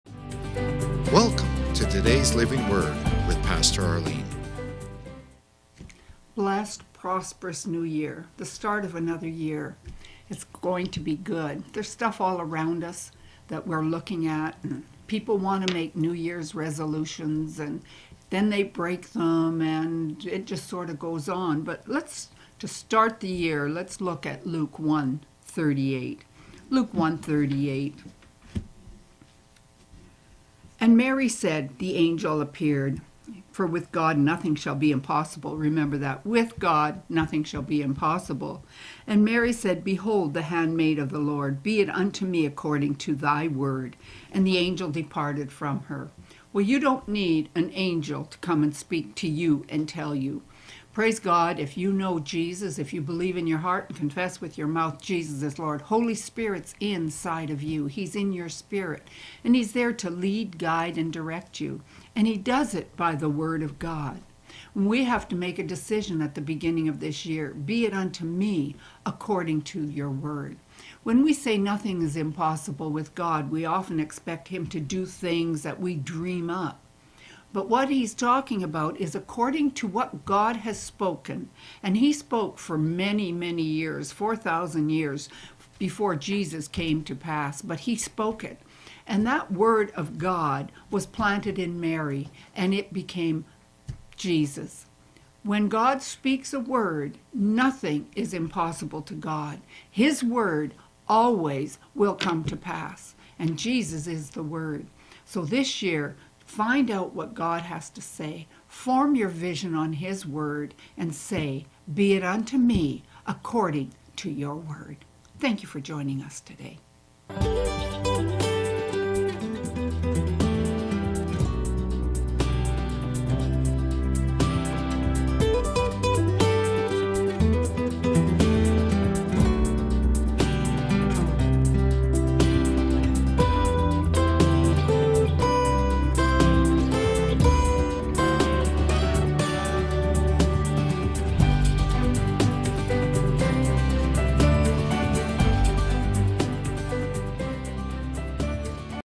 #1 The Living Word for Today
Luke 1:38 Then Mary said, “Behold the maidservant of the Lord! Let it be to me according to your word.” And the angel departed from her.
If you know Jesus, Holy Spirit is in you, and you do not need an angel to speak to you. Holy Spirit leads, guides, and directs us according to the Word of God.
God’s Word will always come to pass, and Jesus is the Word.
Find out what God has to say, form your vision on His Word, and say “be it unto me according to Your Word”.